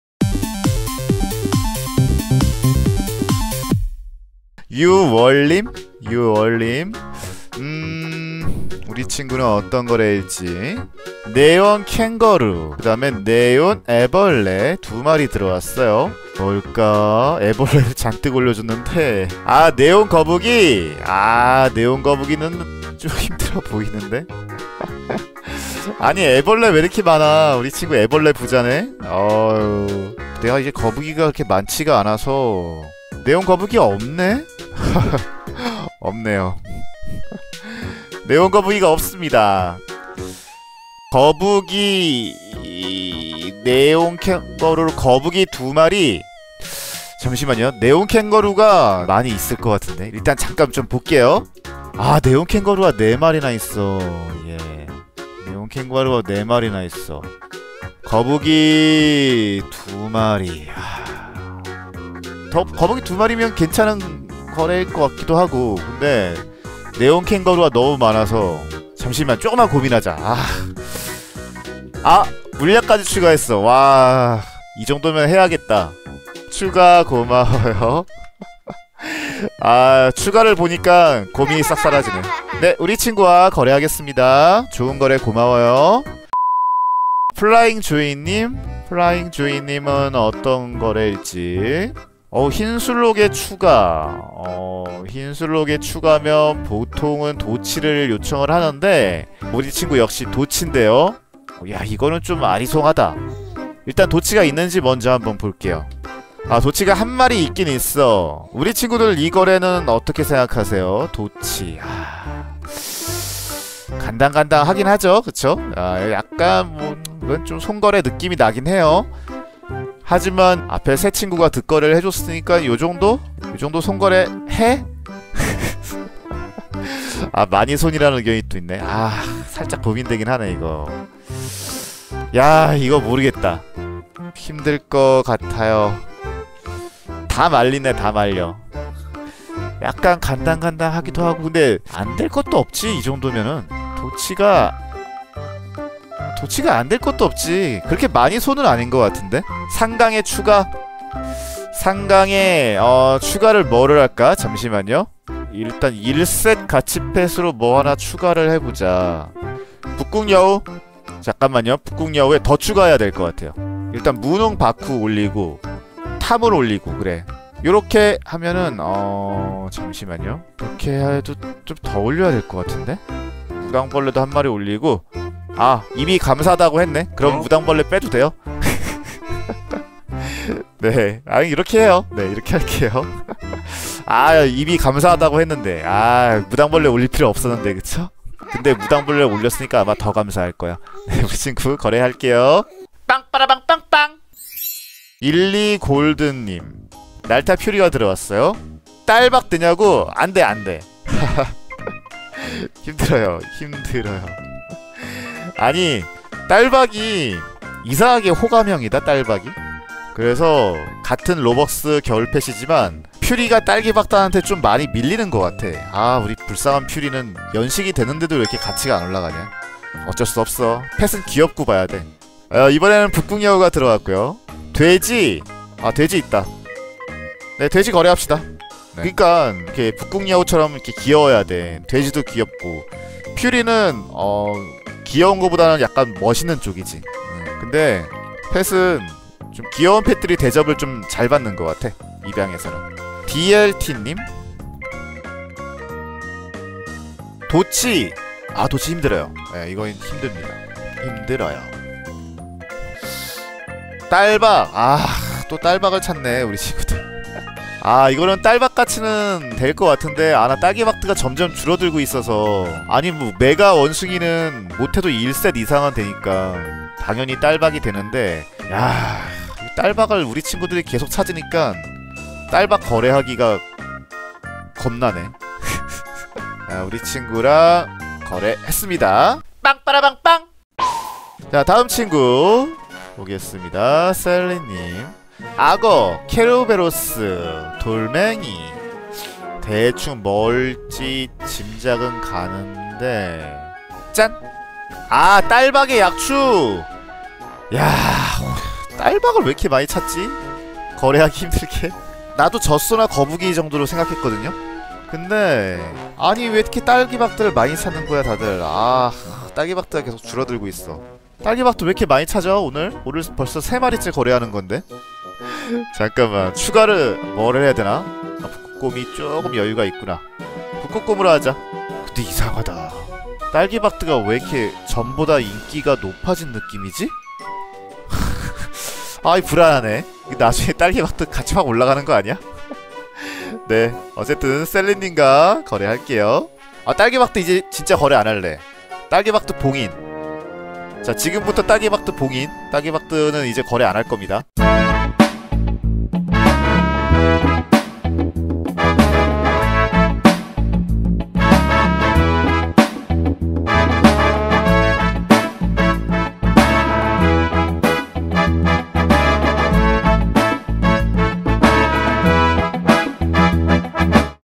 혹시 딸기박드 떡상중인가요?
펫 거래 영상에서 빵신이 말하는 가치가 100프로 정확하지 않아요
가볍게 참고만 해 주시기 바래요
펫 가치를 정확하게 파악하기는 어려운 거 같네요
모두 각자의 기준에 맡게 거래하시길 바래요 ^^